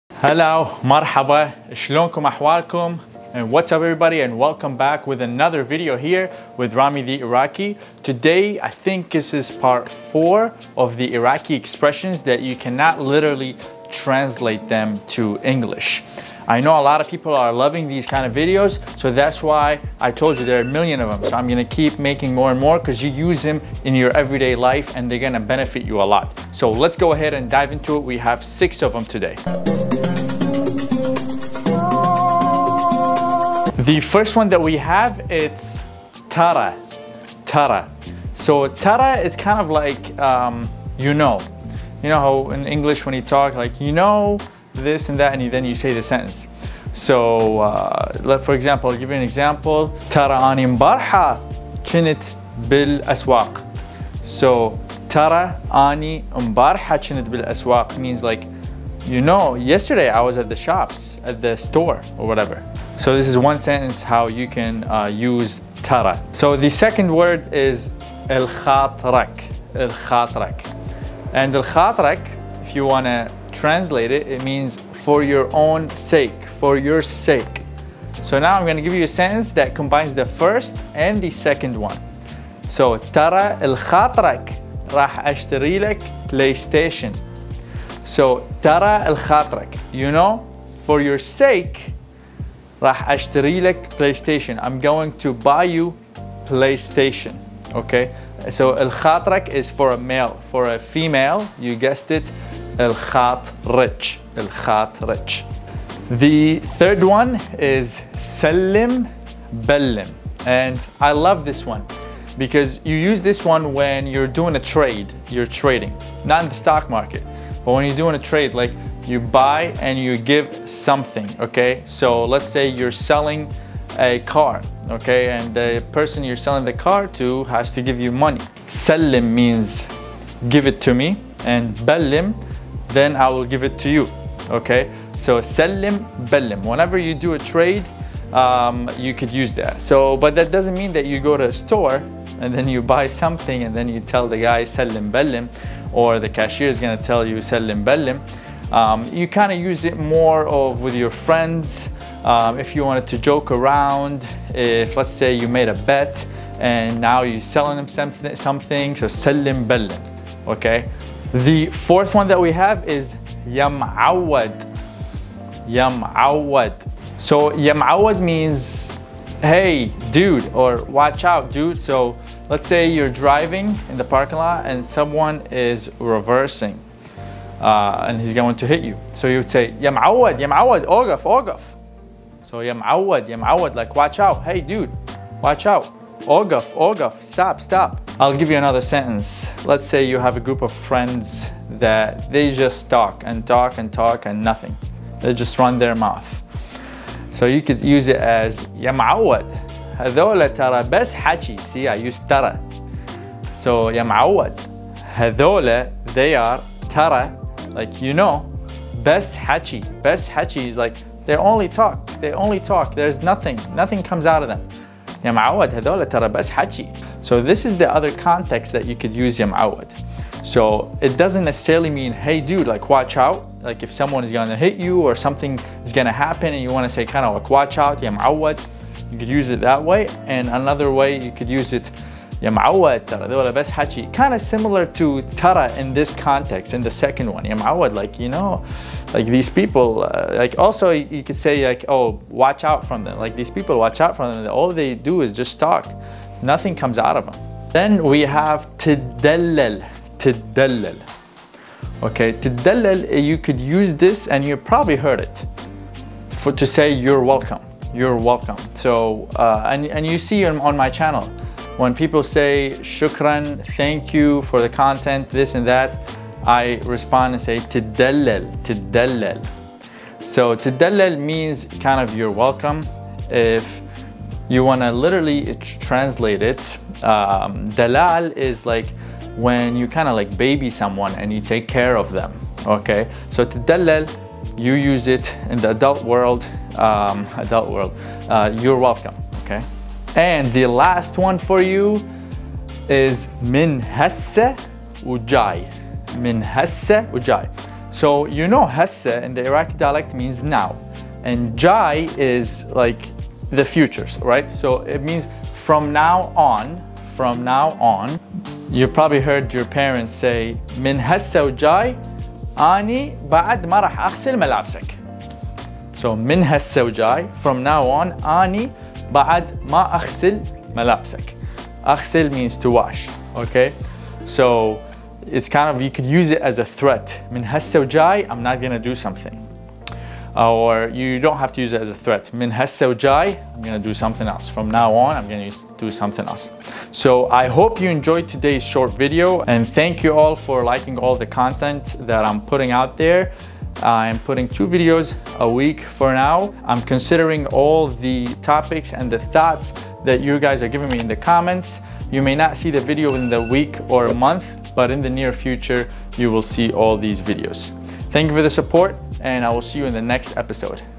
Iraqi Expressions That Are Impossible to Translate 4 - مصطلحات عراقية مستحيل ترجمتها
In this video I will talk about Iraqi Expressions that are impossible to translate in English
في هذ ا الفديو راح نتكلم عن مصطلحات عراقية من الصعب ترجمتها بالانجليزي

Important Videos on the channel

Another Great Channel To Learn Arabic. 100% in Arabic